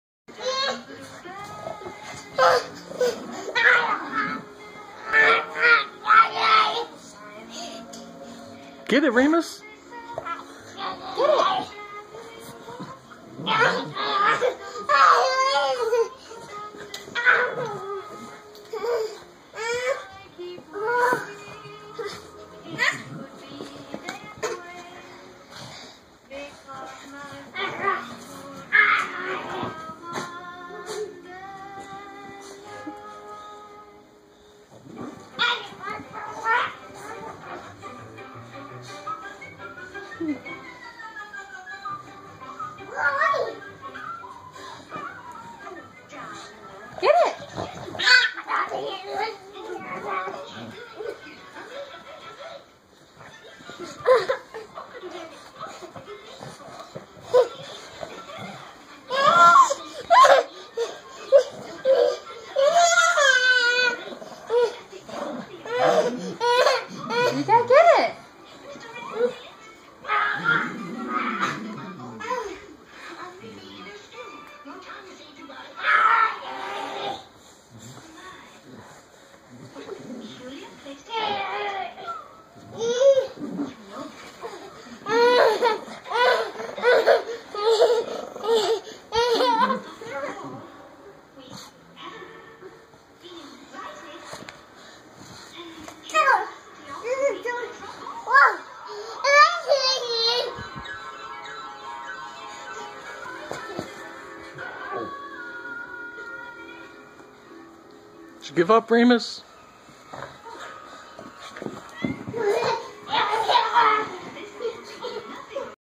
Toddler and pug wrestling over pillow pet.
Toddler and pug fight over pillow pet.